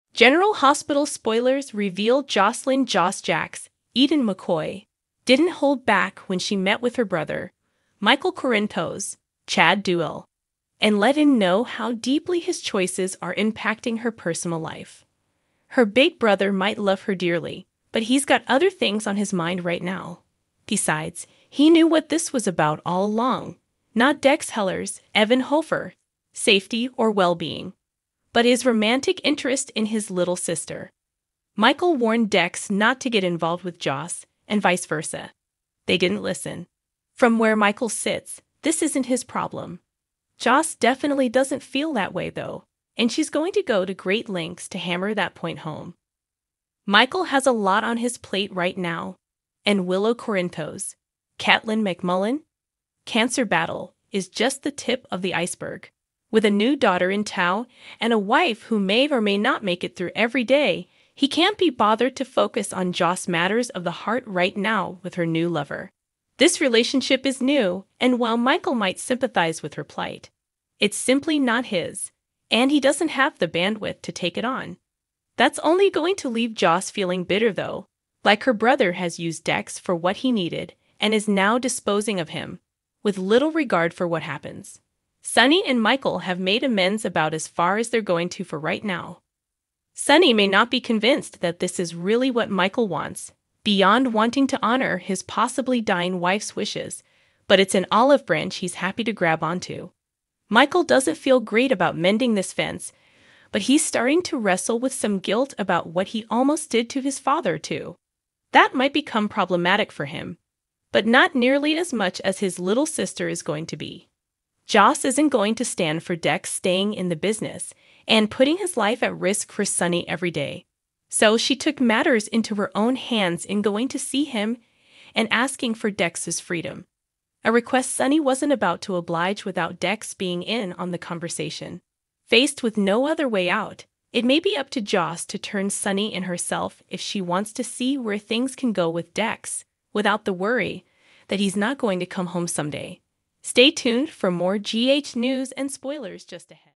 Jaw-Dropping General Hospital Spoiler: Joss Takes Michael to Task Relationship Takes a Shocking
General Hospital Spoilers: Joss Lets Michael Have It — But His Priorities Have Shifted